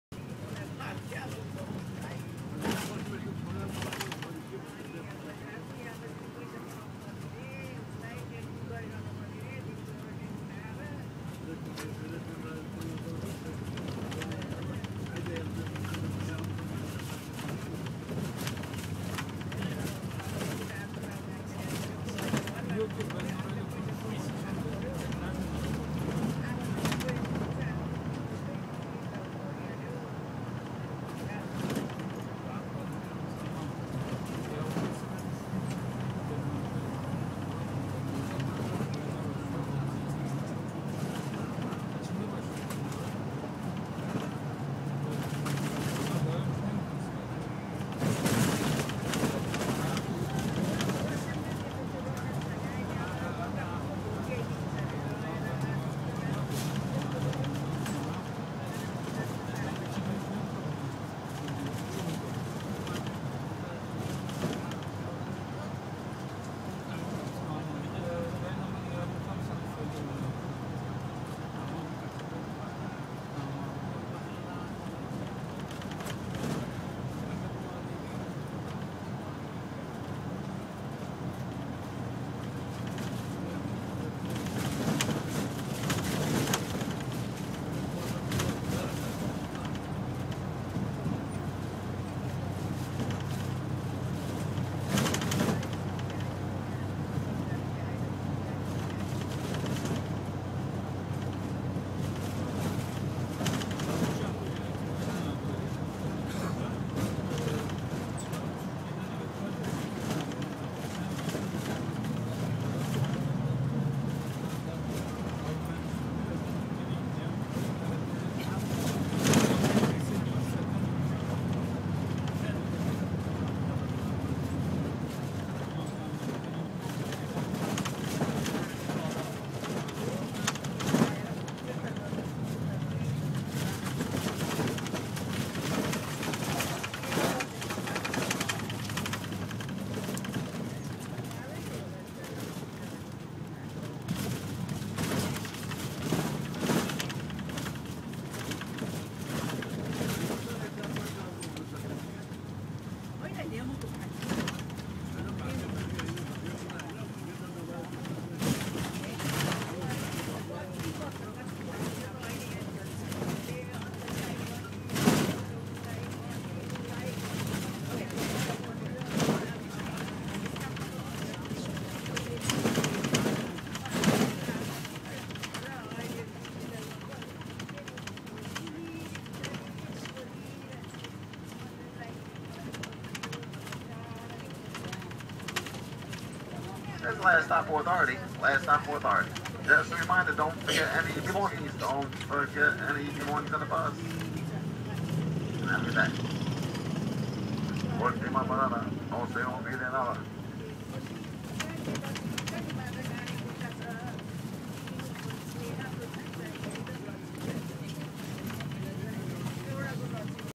Crossing the Lincoln tunnel into NYC
On the bus crossing into Lincoln tunnel.